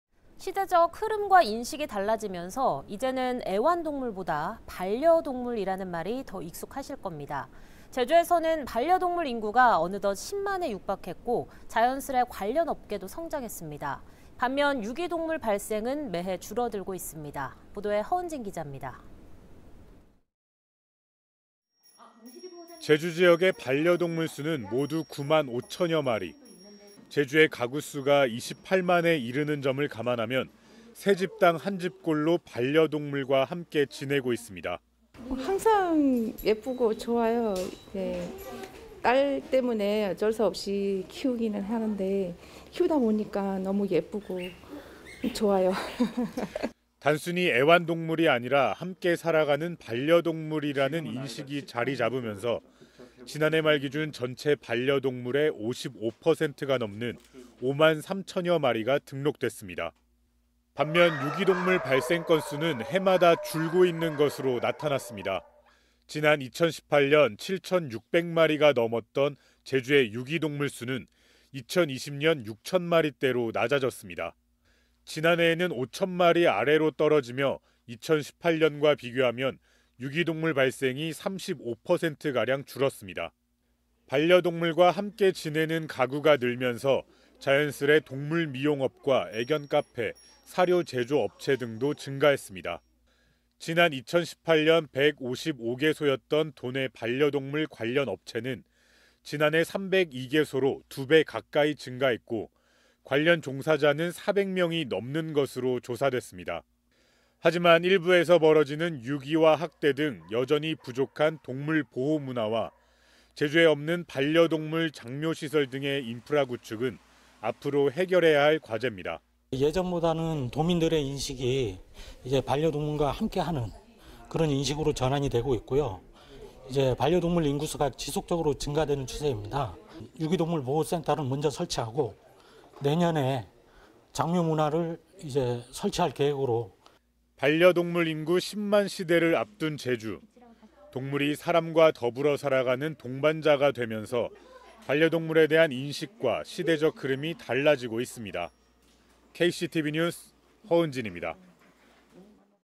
반려동물 10만 시대…유기 줄고 업계 늘고
시대적 흐름과 인식이 달라지면서 이제는 애완동물보다 반려동물이라는 말이 더 익숙하실 겁니다.

제주에서는 반려동물 인구가 어느덧 10만에 육박했고 자연스레 관련 업계도 성장했습니다.

반면 유기동물 발생은 매해 줄어들고 있습니다.